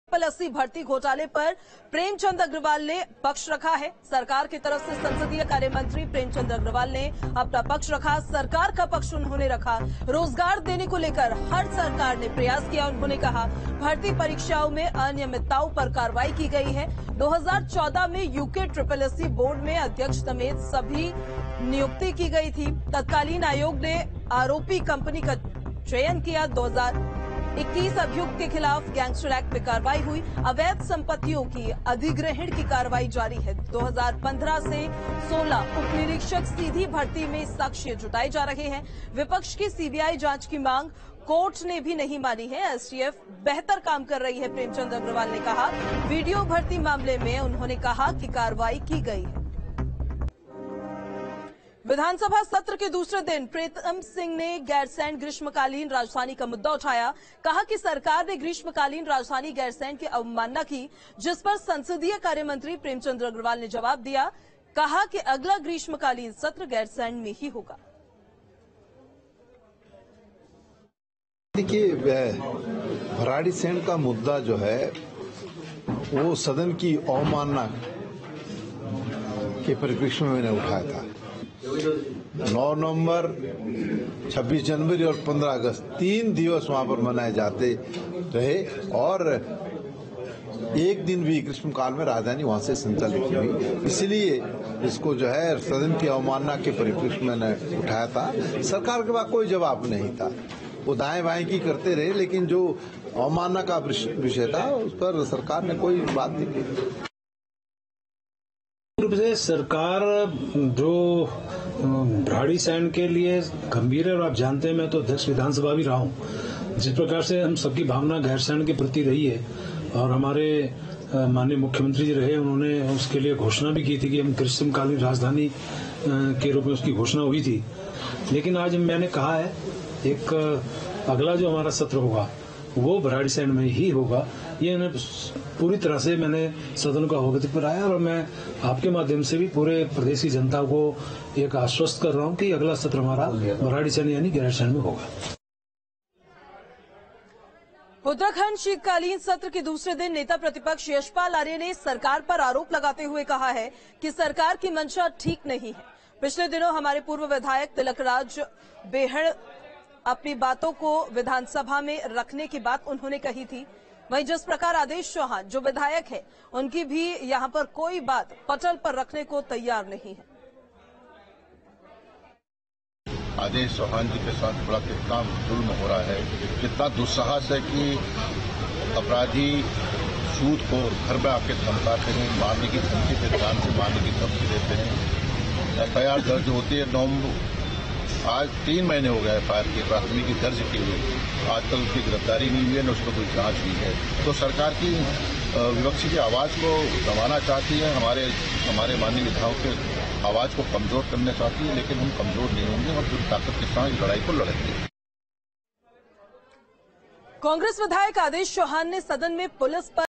Prem Chand Agarwal: भर्ती घोटाले पर प्रेमचंद अग्रवाल ने रखा पक्ष
News State Apps डाउनलोड करें:
Download Android and iOS Apps for more updates: